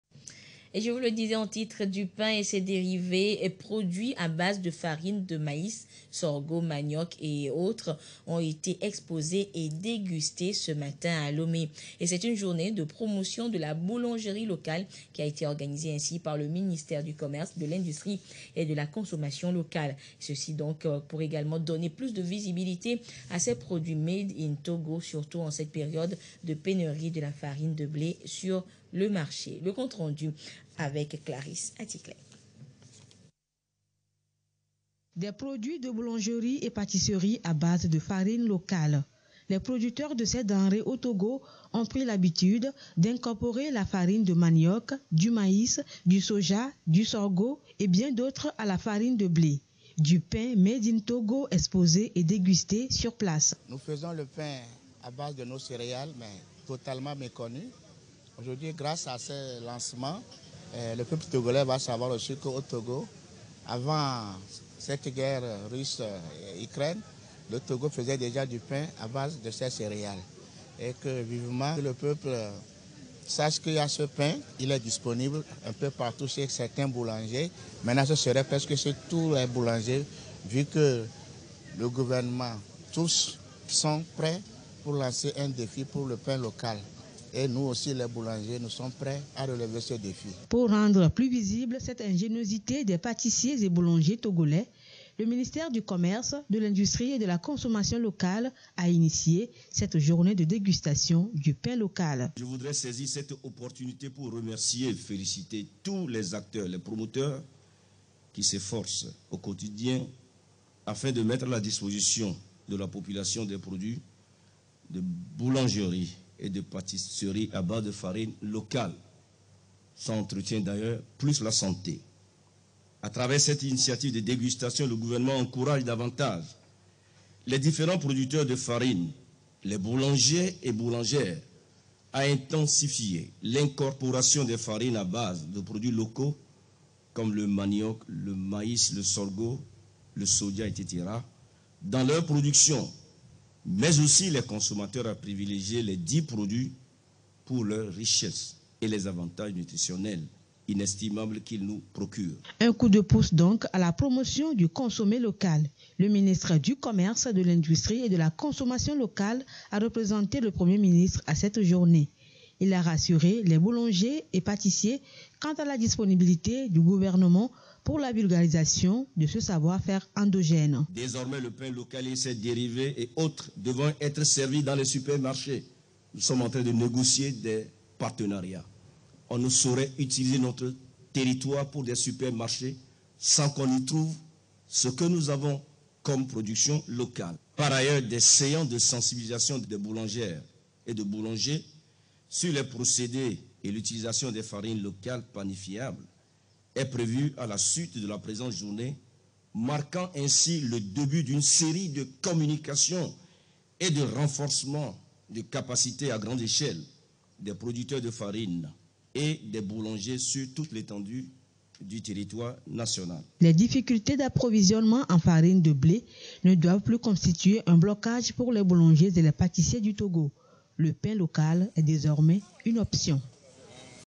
Journée de dégustation de pain local, ce jeudi 07 juillet 2022 à Lomé
Le pain fabriqué souvent à plus de 50 % de la farine de blé, peut aujourd’hui se faire par l’incorporation de plusieurs denrées locales comme la farine de maïs, de sorgho, de manioc et de soja. C’est ce qui a été une fois encore constaté, ce jeudi 07 juillet 2022 à Lomé, à l’occasion d’une « journée de dégustation de pain local » organisée par le ministère du commerce, de l’industrie et de la consommation locale sous le patronage de Madame Victoire TOMEGAH DOGBE, Premier Ministre. Les membres du Gouvernement, les présidents des institutions de la République ainsi que le secteur privé et la société civile se sont mobilisés pour cette journée